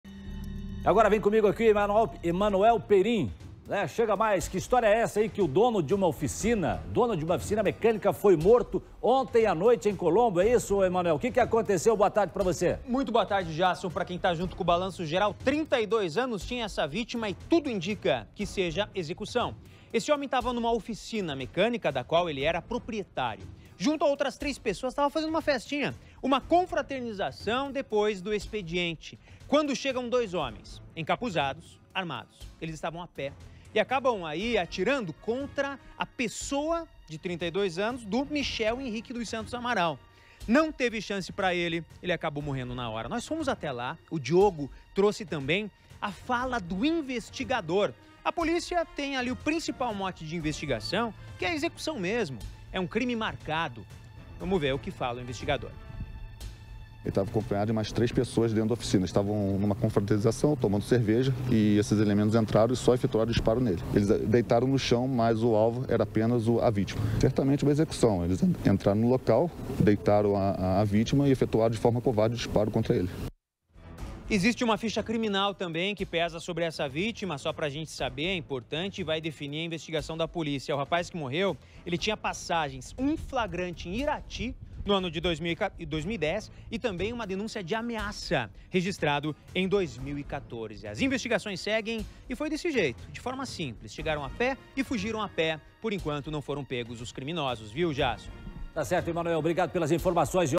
Homens armados invadem oficina mecânica mandam todos para o chão e matam o dono, em Colombo
O homem identificado como Michel Henrique dos Santos Amaral, de 32 anos, foi executado, na noite desta terça-feira (27), na rua Santiago, em Colombo. Segundo informações, o rapaz estava dentro da oficina mecânica em que era proprietário, quando foi alvejado por disparos de arma de fogo por homens encapuzados. Confira todos os detalhes na reportagem!